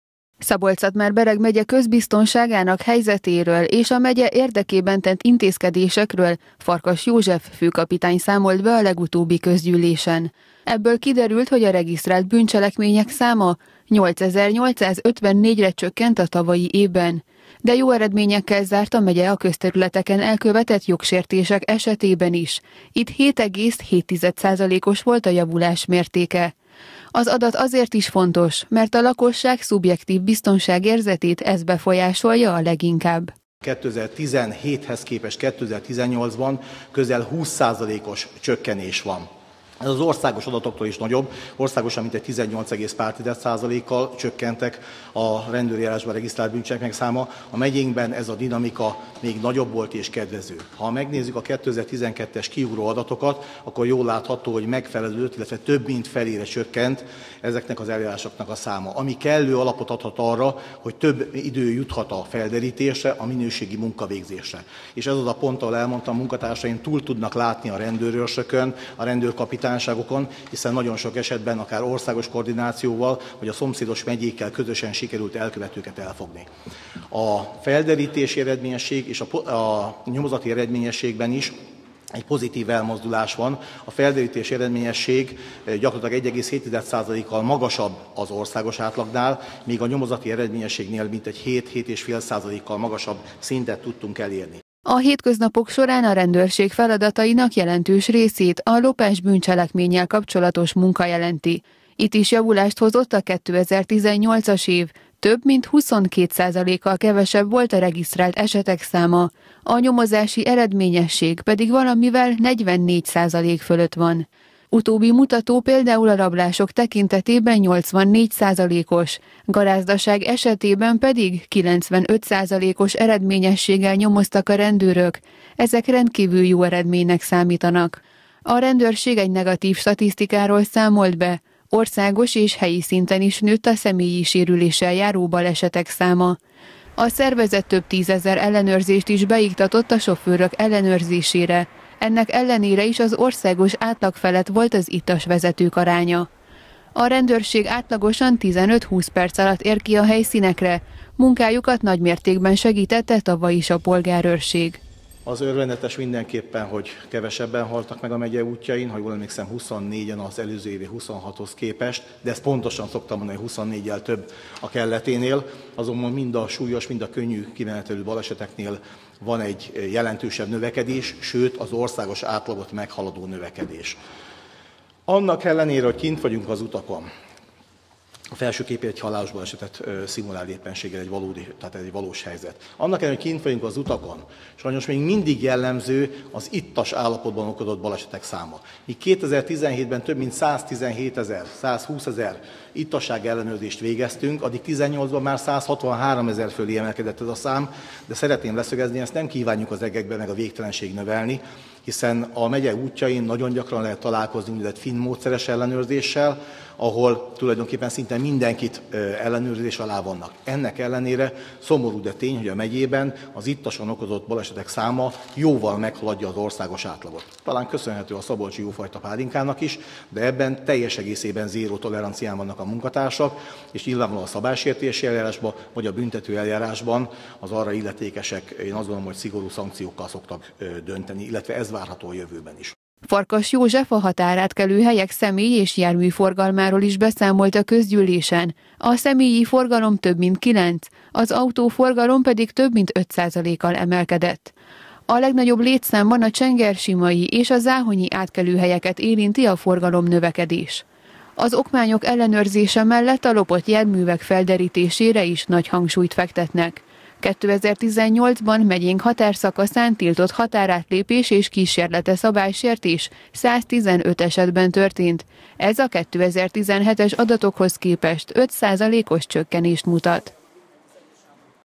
Kevesebb a bűncselekmény Szabolcs megyében - Kölcsey Tv
Mintegy húsz százalékkal csökkent a regisztrált bűncselekmények száma 2018-ban Szabolcs-Szatmár-Bereg megyében az egy évvel korábbi adatokhoz képest.
Kevesebb volt a lopás és a közterületi bűncselekmények száma is, ami főleg kiemelkedő eredmény, hogy a nyomozási eredményesség is nagy mértékben javult. Egy negatív statiszikája van még a szervezetnek, országos és helyi szinten is nőtt a személyi sérüléssel járó balesetek száma.